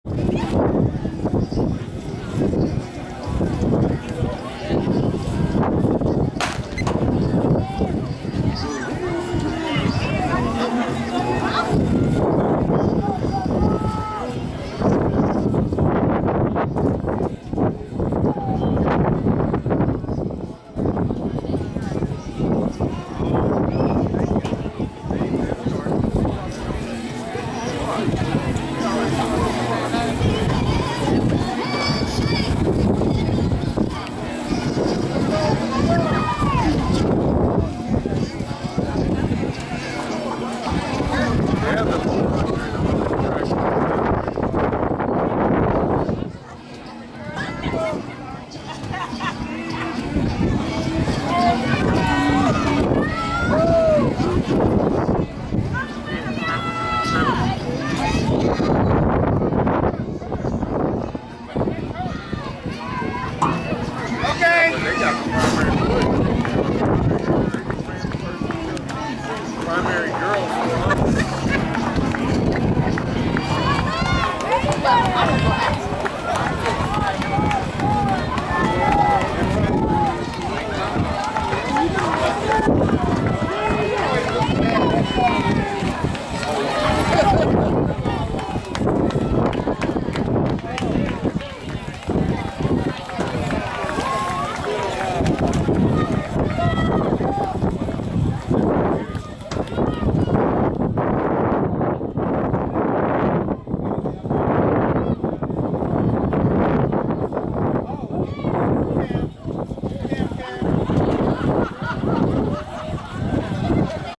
400m - Girls - AAU Primary District Finals - May25 2019
Liz Brasil wins the 400m with 1'25". She won her heat and placed 1st overall.